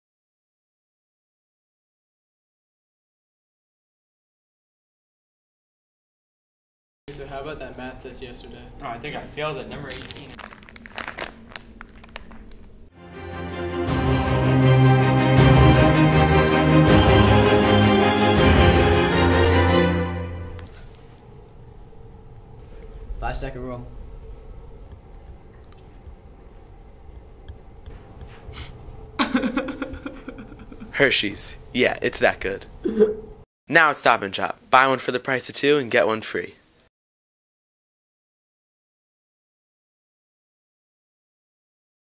DV1: 30-Sec Commercial - Hershey's Chocolate Bar
Digital Video 1: 2011-12
30-Second Commercial
Hershey's Chocolate Bar
Produced by: Eric Goodman-Strycula, Lindsay Weiner
Student produced short for DV1